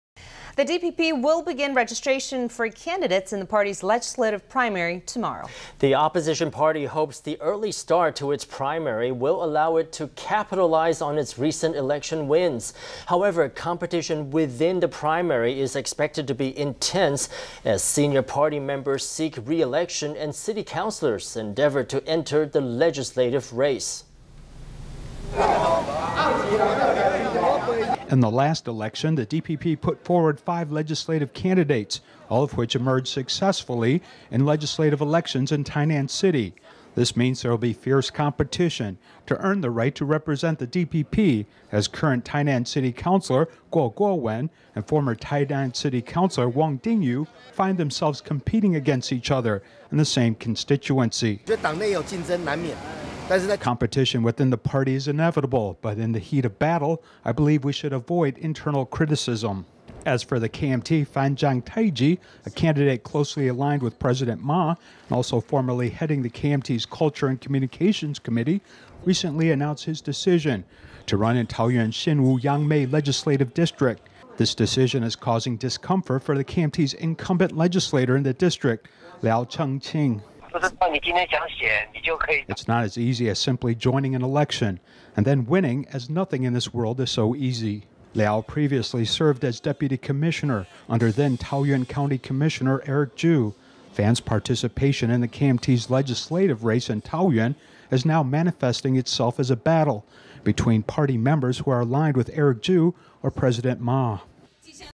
DPP begins registration for legislative primaries tomorrow
The DPP will begin registration for candidates in the party’s legislative primary tomorrow. The DPP hopes the early start to its primary will allow it to capitalize on its recent election wins. However, competition in the DPP primary is expected to be intense as party veterans seek reelection and city councilors seek to advance to the next level.In the last election, the DPP put forward 5 legislative candidates all of which emerged successfully in legislative elections in Tainan City. This means there will be fierce competition to earn the right to represent the DPP as current Tainan City Councilor Kuo Kuo-wen and former Tainan City Councilor Wang Ding-yu find themselves competing against each other in the same constituency.Wang Ding-yuFormer Tainan City CouncilorCompetition within the party is inevitable, but in the heat of battle, I believe we should avoid internal criticism.  As for the KMT, Fan Chiang Tai-chi, a candidate closely aligned with President Ma and also formerly heading the KMT’s Culture and Communications Committee, recently announced his decision to run in Taoyuan’s Xinwu Yangmei legislative district. This decision is causing discomfort for the KMT’s incumbent legislator in the district, Liao Cheng-ching.Sound of KMT Legislator Liao Cheng-ChingIt’s not as easy as simply joining an election and then winning as nothing in this world is so easy.Liao previously served as deputy commissioner under then Taoyuan County Commissioner Eric Chu. Fan’s participation in the KMT’s legislative race in Taoyuan is now manifesting itself as a battle between party members who are aligned with Eric Chu or President Ma.